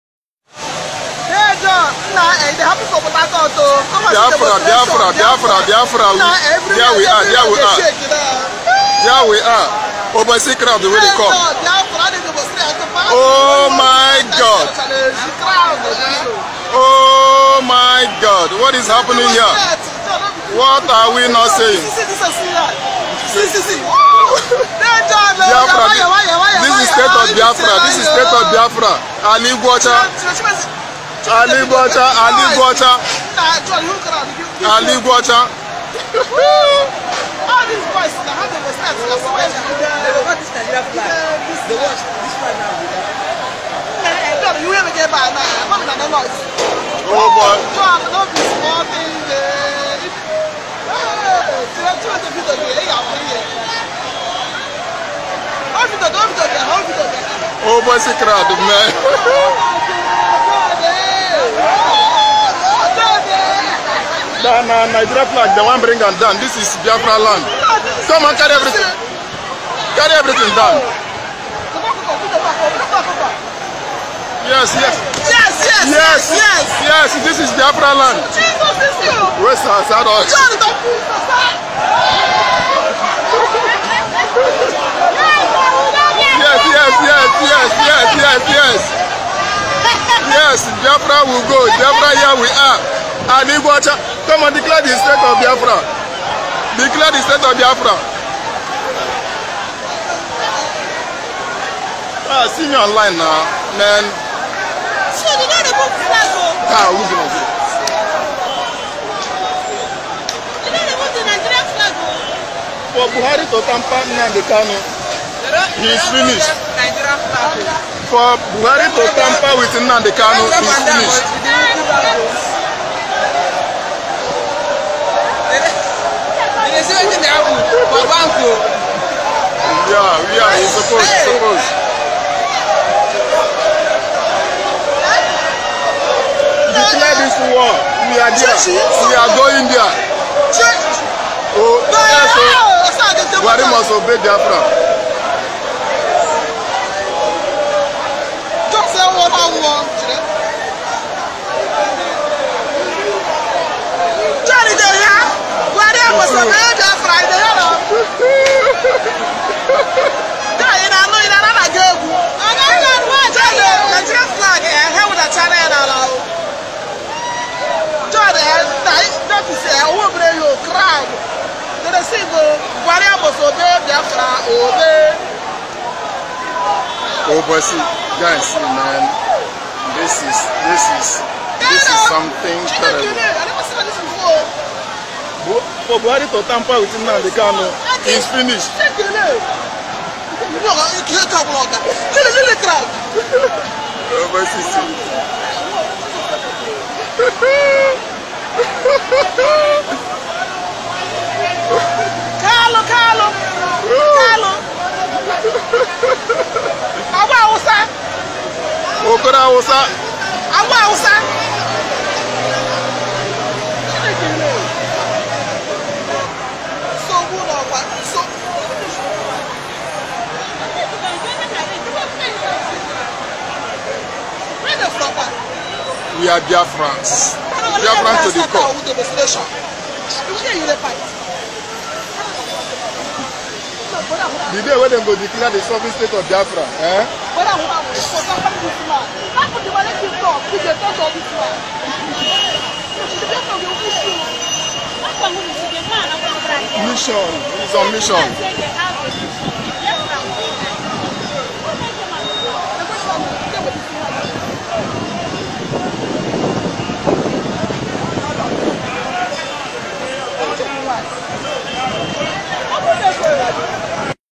igweocha porthacourt in a serious protest over mazi NNAMDI KANU's arest
72 hour is not an empty threat...